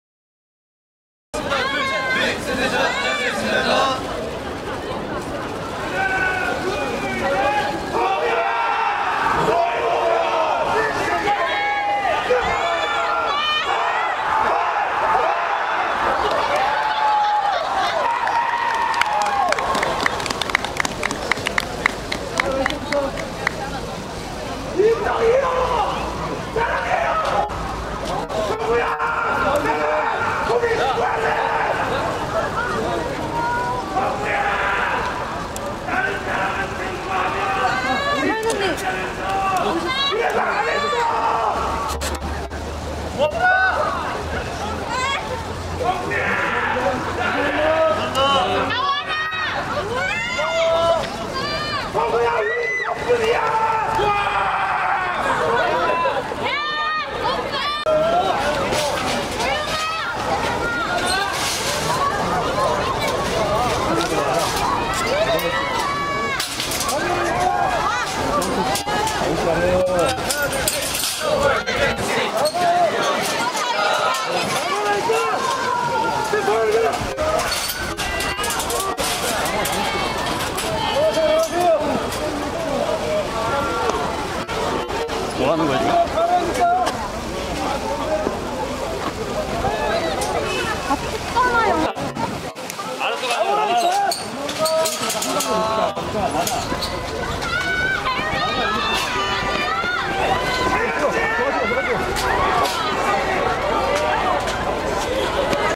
[뉴스인스타] SF9, NCT127 '팬들이 너무 많은 꽃미남들' (180330 뮤직뱅크 출근길)
[뉴스인스타] '' (180330 뮤직뱅크 출근길)

180330 뮤직뱅크 리허설 출근길 풍경 입니다.

30일 '뮤직뱅크'에는 씨엘씨(CLC), 갓세븐(GOT7), NCT 127, SF9, Stray Kids, W24, 워너원 (Wanna One), 김연자, 동방신기, 르씨엘 (Le Ciel), 몬스타엑스, 사무엘 (Samuel), 설하윤, 아이스, 에이지엠 (AZM), 에이프릴, 자이언트 핑크, 천단비가 출연합니다.